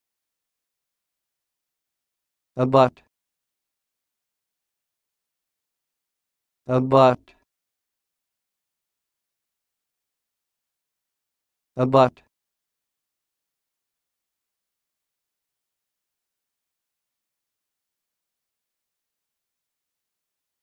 Learn how to say this word: "Abut"
This is the definition of "Abut":
v. i.  To project; to terminate or border; to be contiguous; to
   meet; -- with on, upon, or against; as, his land abuts on the road.